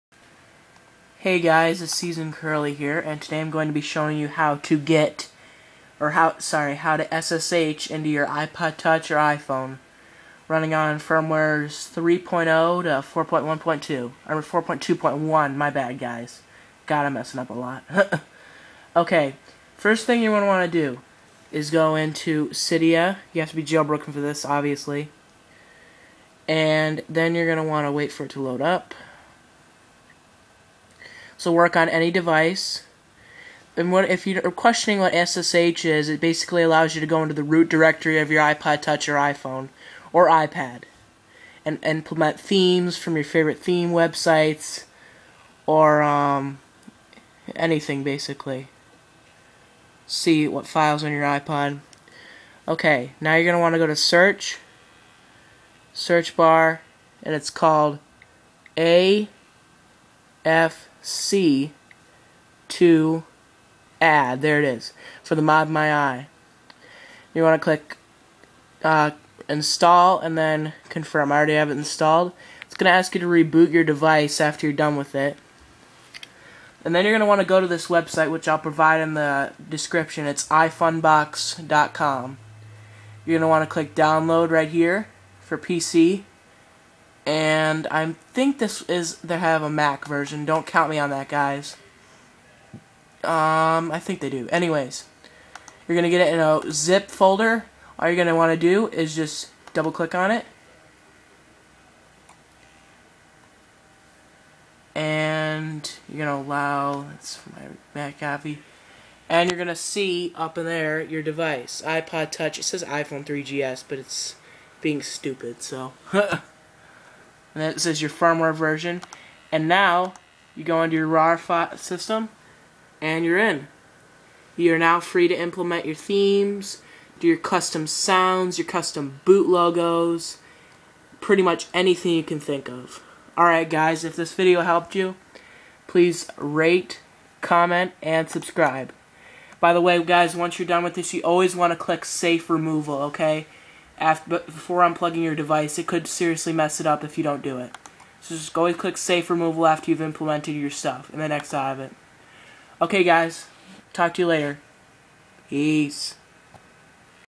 How to SSH into iPod Touch or iPhone
Hey guys in this video I show you how to ssh or get into your iDevices root directory.